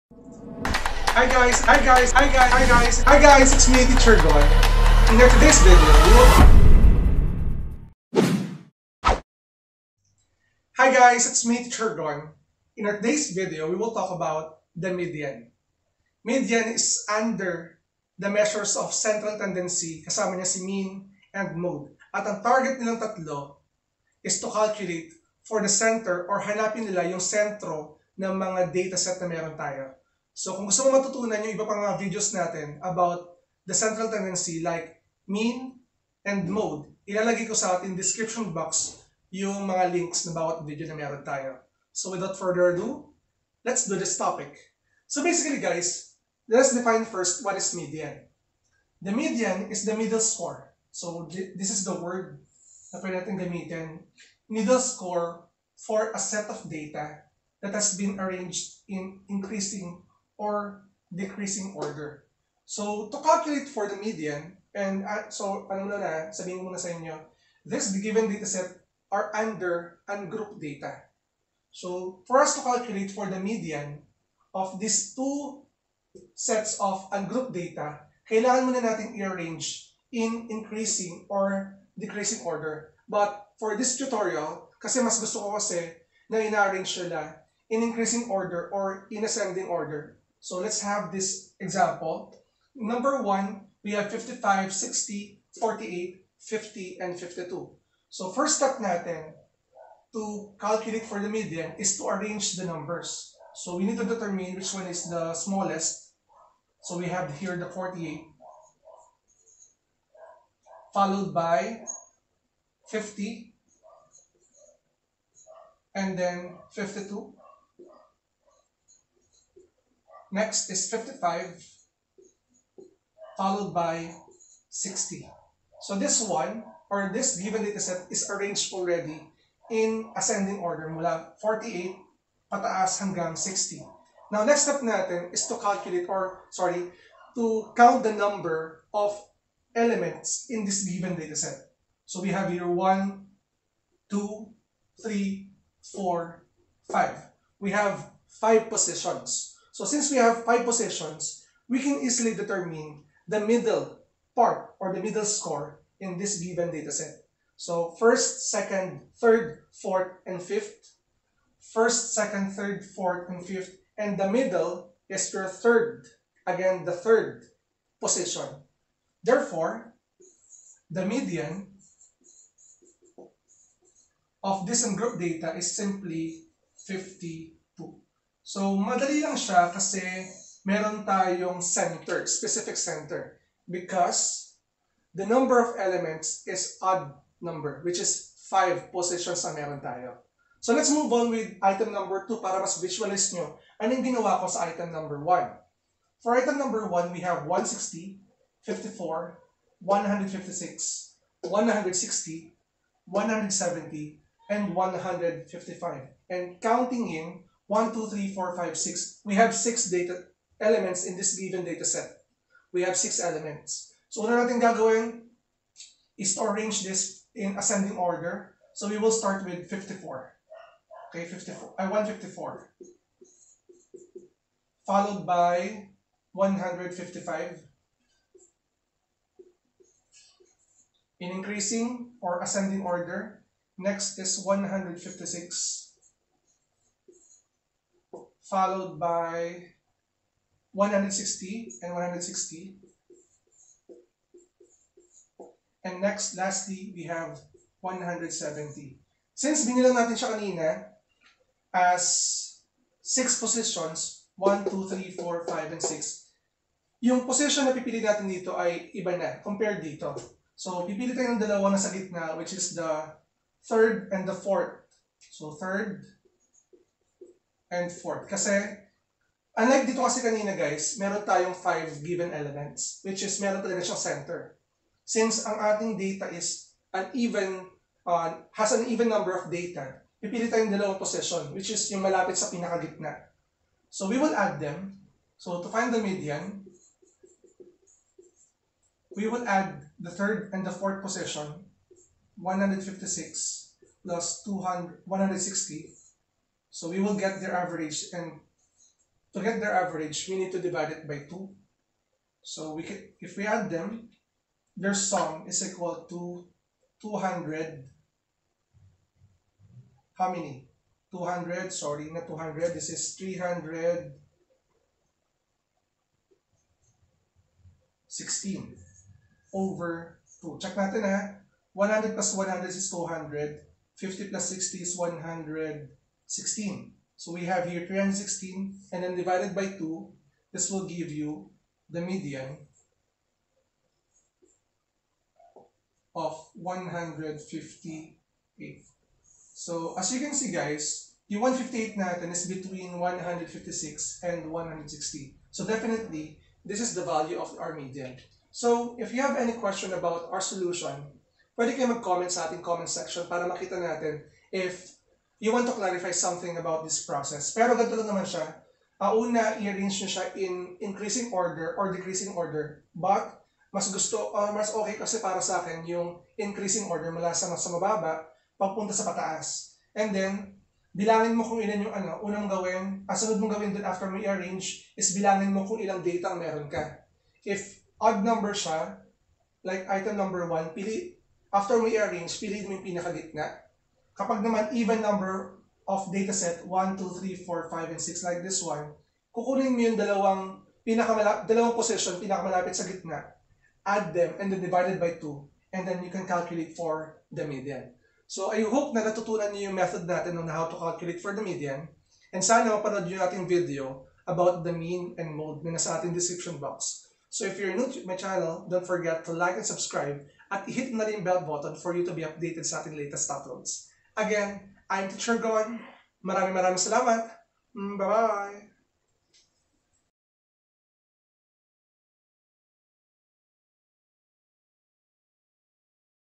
Median - Measures of Central Tendency
@MathTeacherGon  will demonstrate how to find the median of ungrouped data in Statistics.